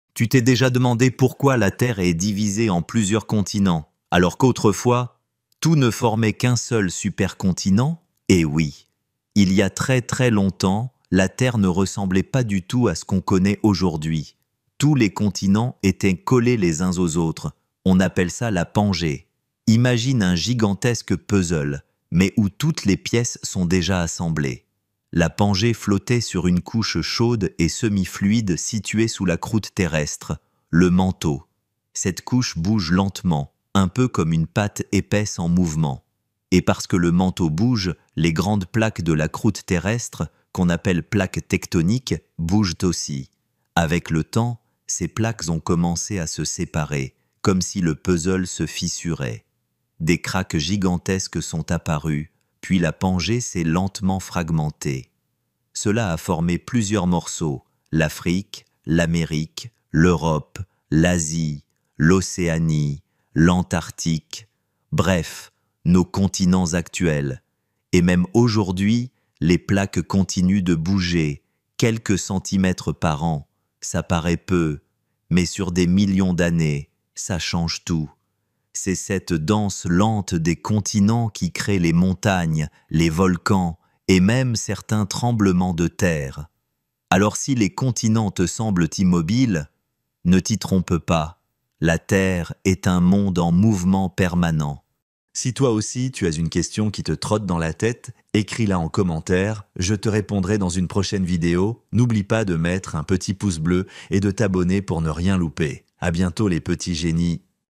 🌍 Pourquoi les continents se sont séparés ? L’histoire de la Pangée expliquée !
Tu sais que les continents n’ont pas toujours été séparés ? Il y a des millions d’années, ils formaient un seul supercontinent : la Pangée !

Dans ce short éducatif, découvre comment les plaques tectoniques ont fracturé la Terre, pourquoi les continents dérivent encore aujourd’hui, et comment ce mouvement crée montagnes, volcans et tremblements de terre. Une explication simple, rapide et passionnante pour comprendre notre planète !